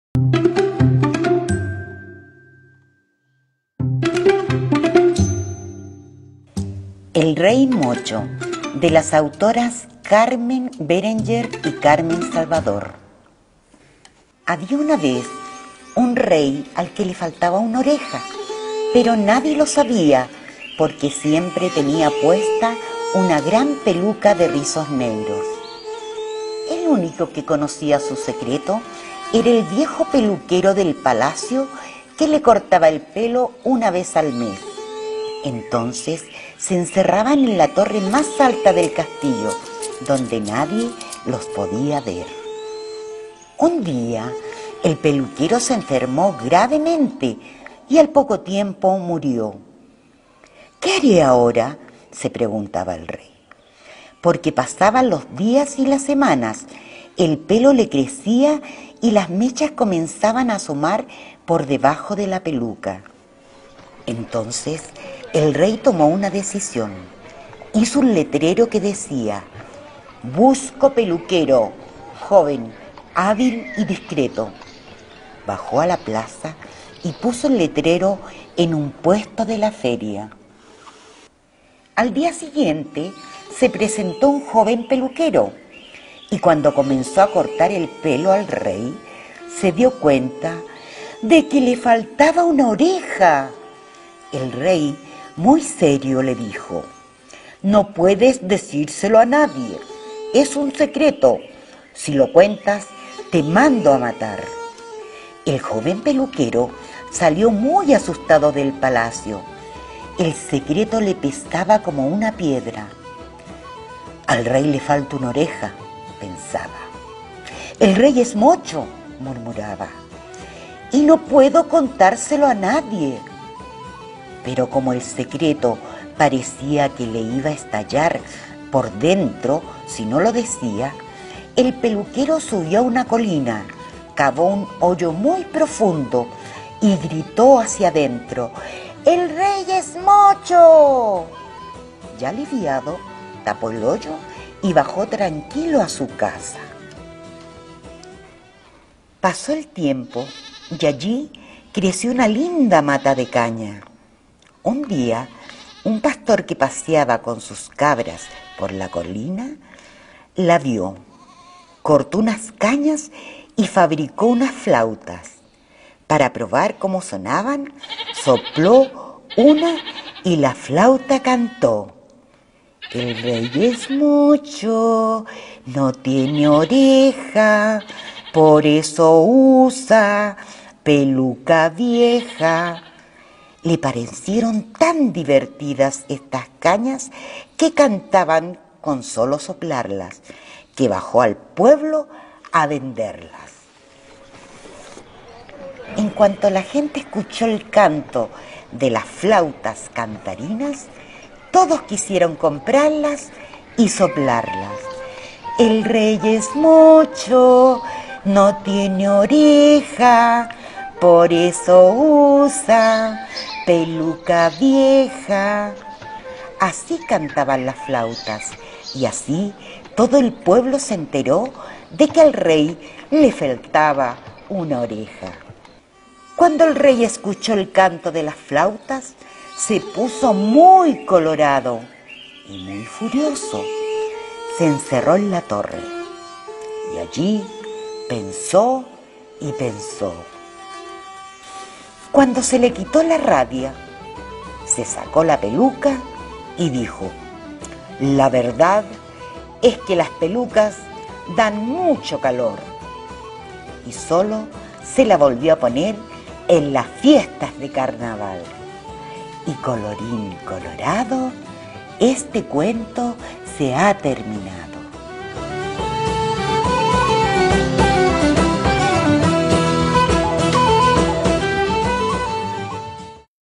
Cuentacuentos "El Rey Mocho"
Invitamos a toda la familia a seguir disfrutando de los “Cuentacuentos Online” preparados por el Área de Bibliotecas de la Corporación de Cultura y Turismo de Calama. En esta oportunidad les dejamos "El Rey Mocho". ¡Qué lo disfruten!